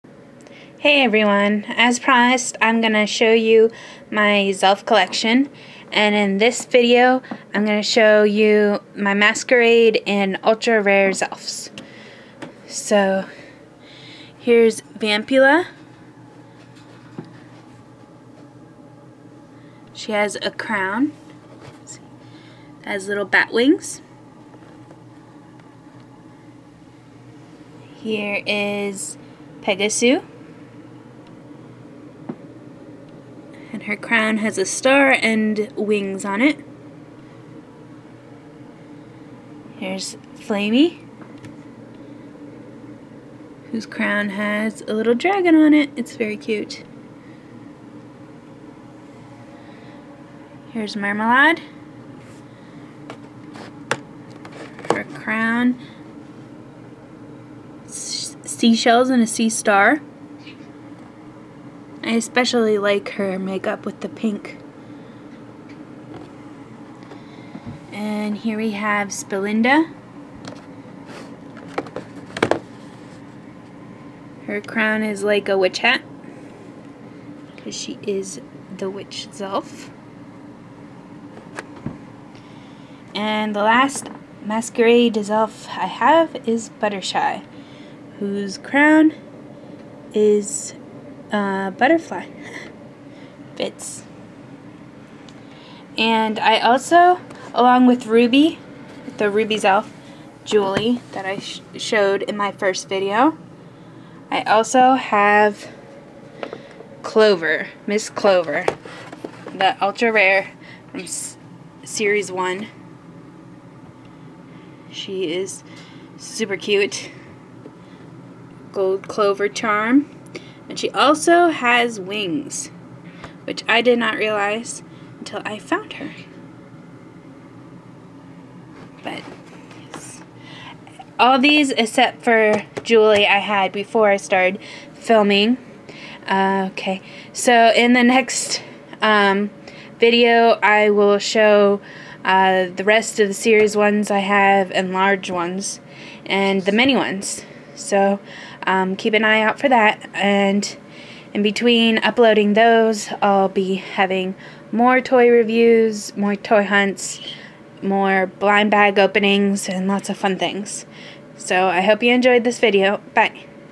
Zelf Collection-Miss Clover, Jewely, and Masquerades
Showing off my ultra rare zelfs and the masquerade line.